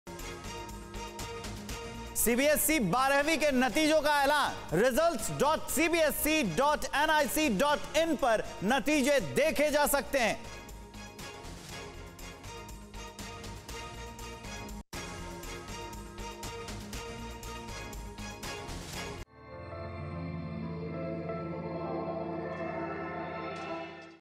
CBSE Result 2022 : CBSE 12वीं के नतीजों का ऐलान, एक बार फिर लड़कियों ने बाजी मारी
CBSE 12वीं के नतीजों का हुआ ऐलान,एक बार फिर लड़कियों ने बाजी मारी,लड़कियों का पास प्रतिशत 94.54,results.cbse.nic.in पर नतीजे देख सकते हैं